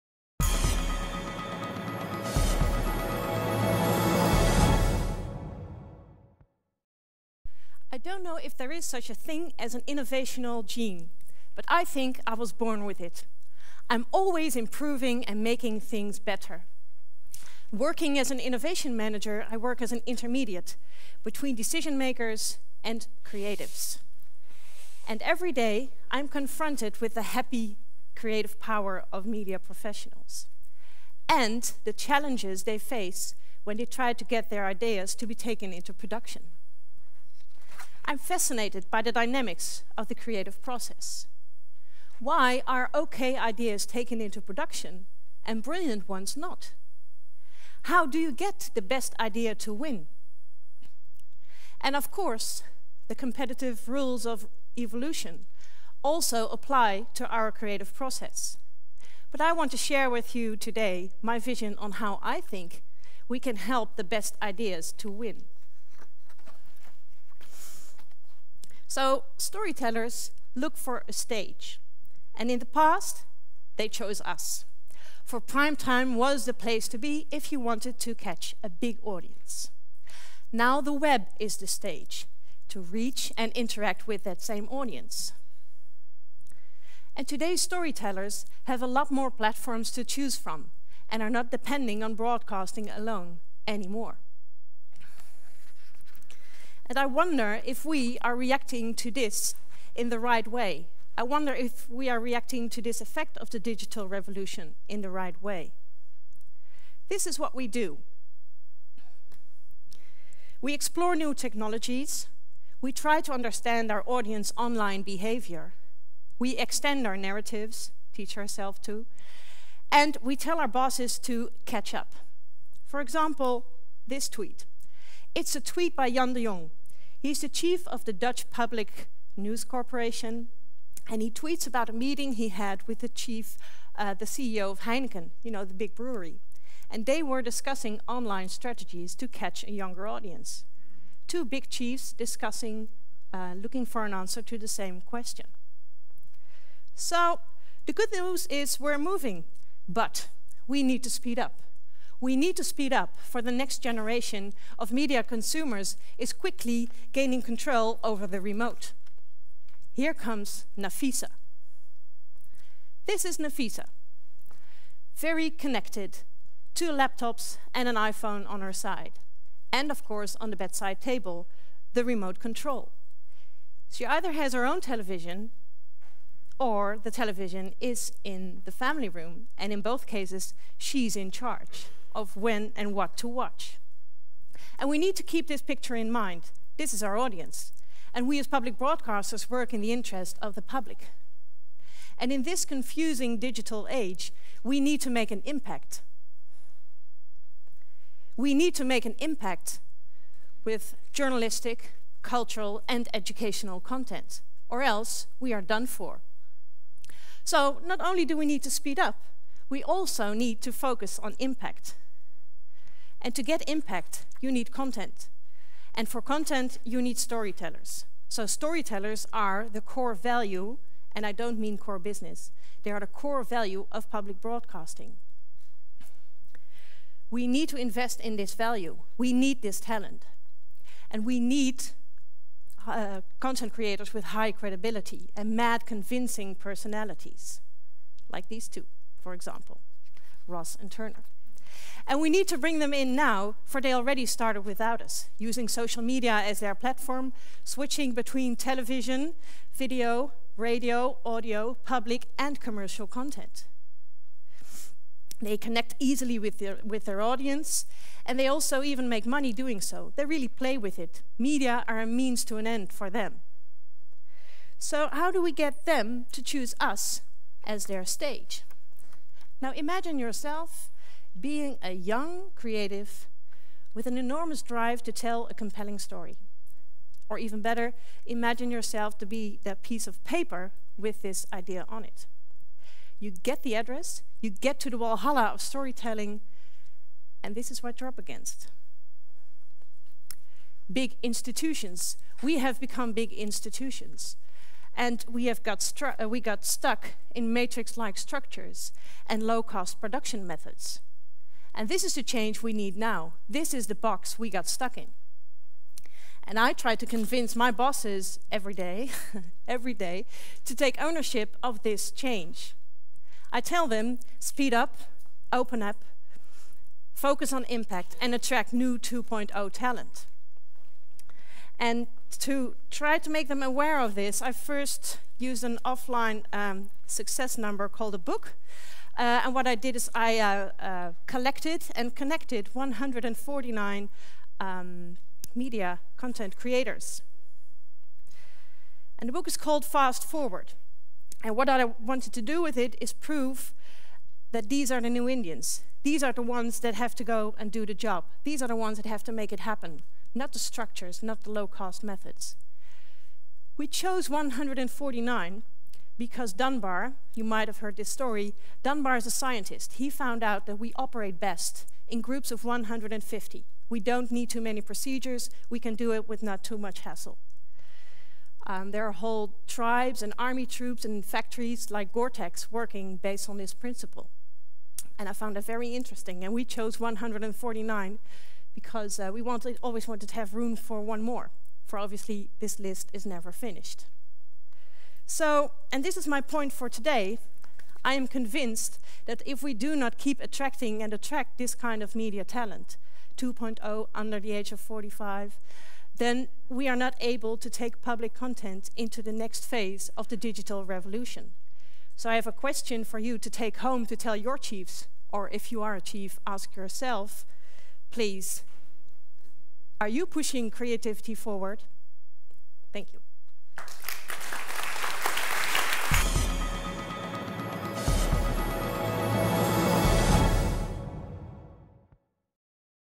Out of the Box: Marieke Hermans at TEDxTransmedia2011
Marieke Hermans is a creative media professional with a clear vision of innovation in media content. As Innovation Manager at the Dutch Public Broadcasting (NPO) she directs the NPOX network for over 2500 media professionals initiating and organizing more than 200 workshops and an annual festival. As project manager of Eurovision TV Lab she contributes to the innovation of video formats, content and audience participation of public content for Nederland3.

In TEDxTransmedia she defies us to think out of the box, in order to help the best -not the average- ideas to get to production and to the audience.